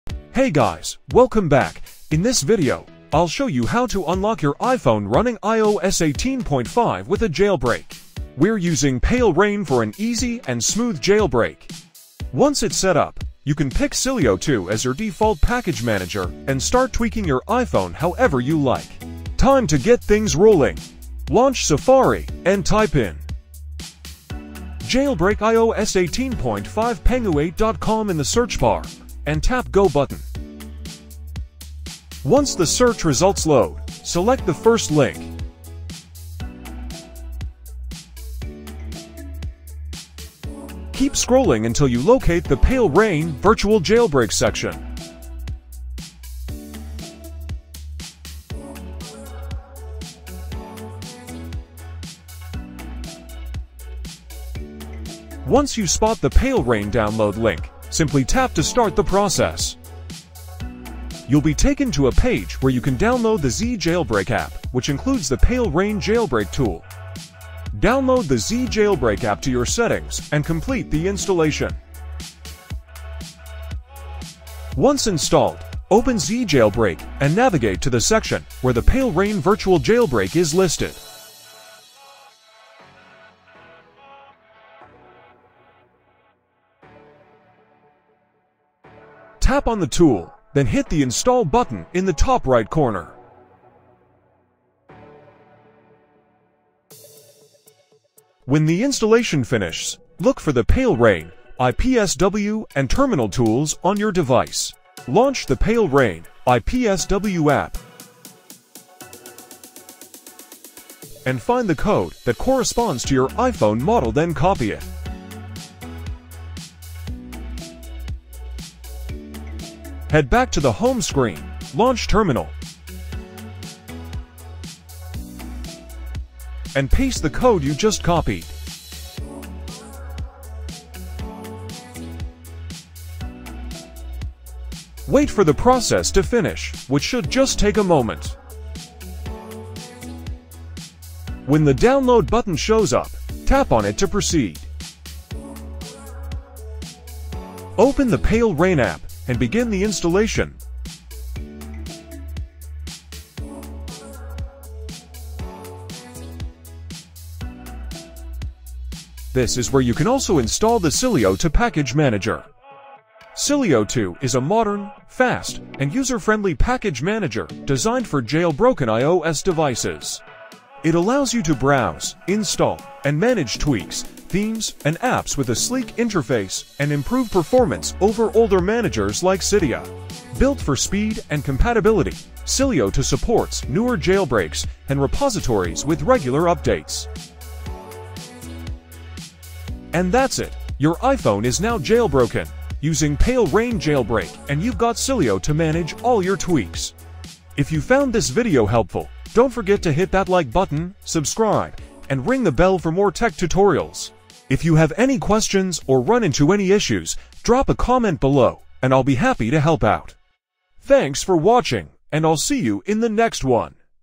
🔓 Palera1n Jailbreak for iOS 18.5 & iPadOS 18.5 – Install Sileo Easily (No PC Required!)
Looking to jailbreak iOS 18.5 or iPadOS 18.5? The Palera1n Online Jailbreak Tool is now the ONLY working jailbreak solution for iOS 18.5 devices. With this method, you can install the Sileo package manager quickly and easily — all without a computer!

✅ Fully supports iPhone & iPad running iOS/iPadOS 18.5
⚡ No need for the complicated Palera1n PC tool – it’s not supported on iOS 18.5
🛠️ Sileo is now 100% compatible with iOS 18.5 via the Palera1n Online Jailbreak
🌐 Just visit the Pangu8 iOS 18.5 Jailbreak Page, install the Palera1n Online tool, run the jailbreak, and enjoy full Sileo access!

🔐 Other jailbreak tools DO NOT support iOS 18.5, making Palera1n Online the best and only choice for jailbreak lovers.

📥 Get started now and unlock powerful customization with Sileo, jailbreak tweaks, themes, and more!